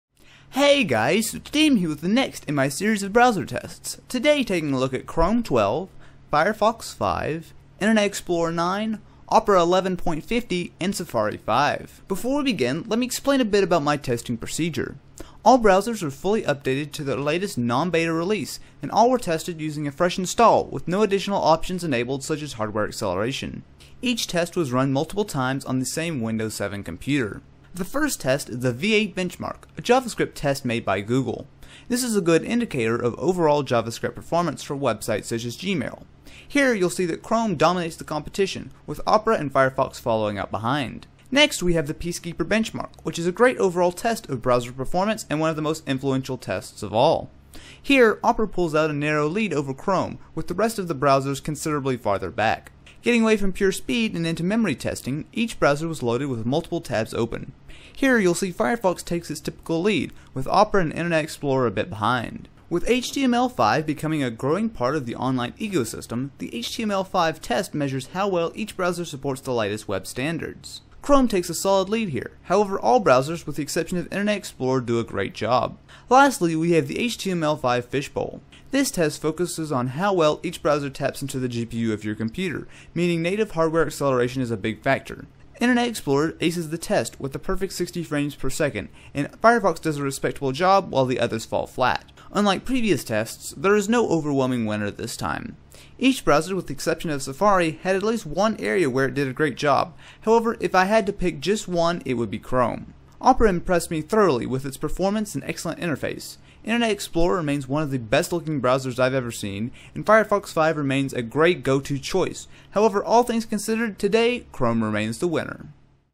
Browser Test: Chrome 12 vs Firefox 5 vs Internet Explorer 9 vs Opera 11.50 vs Safari 5
I compare each of the 5 main browsers to see which is the fastest.

In this video I compare Chrome 12 vs Firefox 5 vs Internet Explorer 9 vs Opera 11.50 vs Safari 5 to see which is fastest at Javascript, general web browsing, HTML5 compliance, memory usage and GPU hardware acceleration. Each browser has their own pros and cons as well as special features however here I just look at the raw benchmarks to determine the winner.

The benchmarks I use are the V8 Benchmark Version 6, Peacekeeper, how much memory each browser takes up as measured from Task Manager, HTML5 Fishbowl and the HTML5 Test. Each is run multiple times on the same Windows 7 computer to ensure a fair fight.